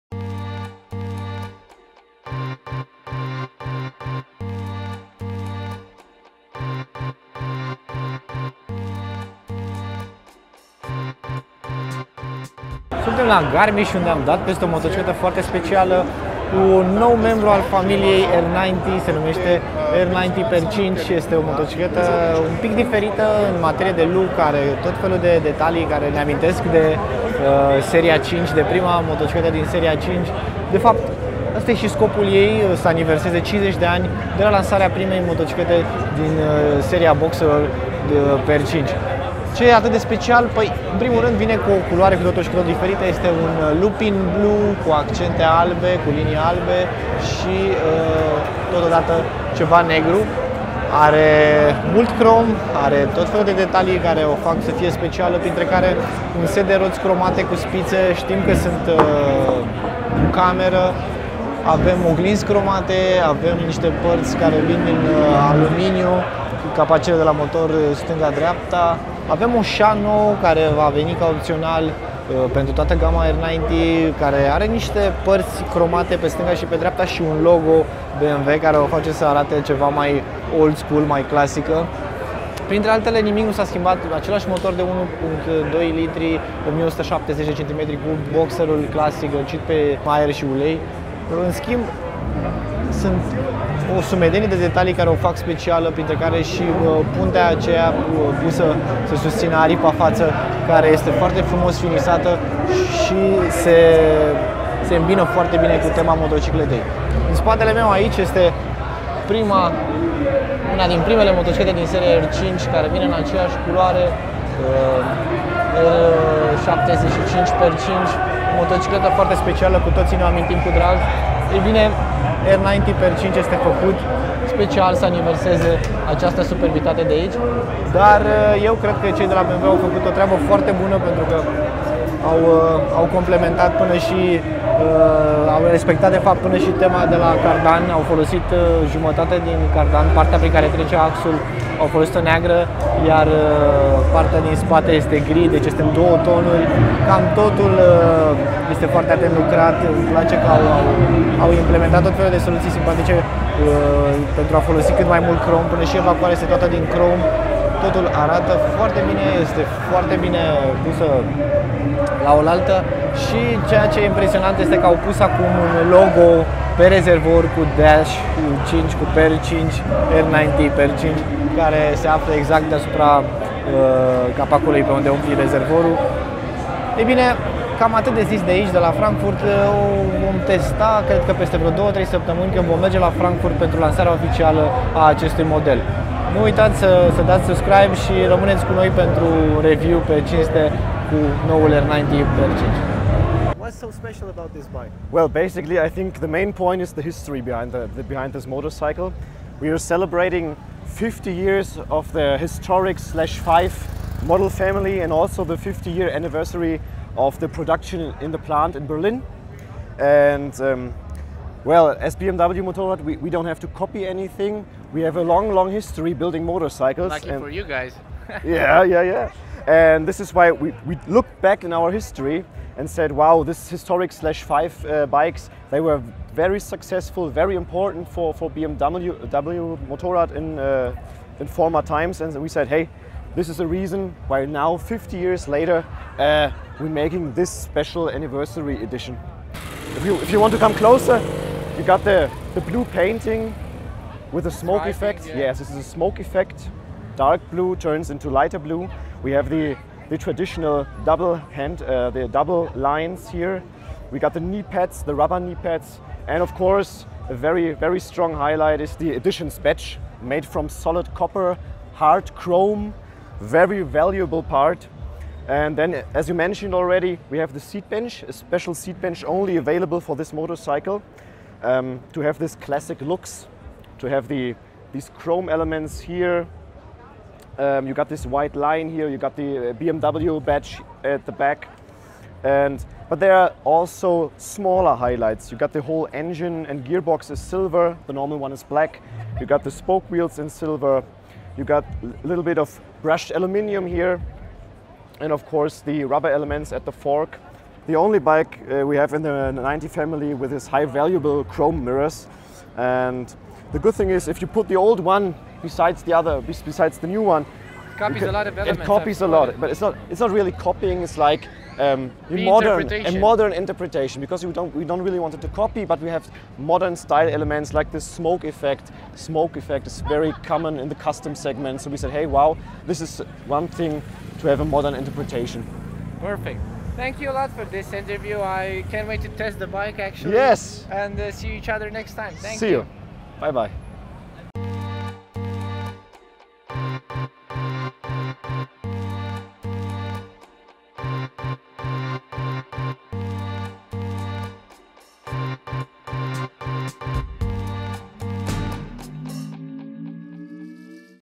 BMW R nineT/9. Model Aniversar - Walkaround, primele impresii
Faceti cunostinta cu cel mai nou membru al familiei R nineT, lansat la BMW Motorrad Days, în Garmisch-Partenkirchen, Munchen. Modelul /5 celebrează 50 de ani de la apariția seriei omonime și 50 de ani de startul producției în uzina din Berlin Spandau. Abia am revenit de la Garmisch și vă aducem primele impresii.